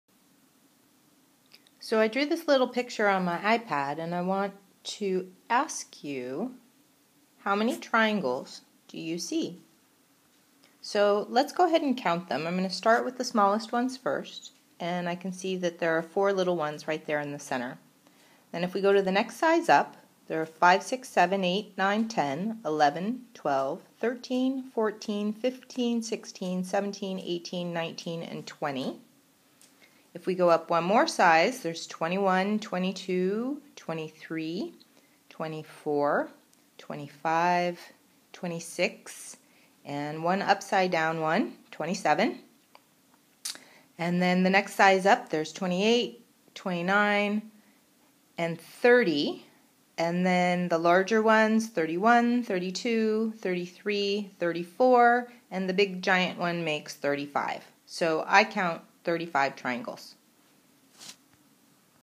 How many triangles? A Doceri Math Puzzle
We solved this puzzle using Doceri to visually keep track of the triangles as we counted them. What answer do you come up with? Feel free to post a video response!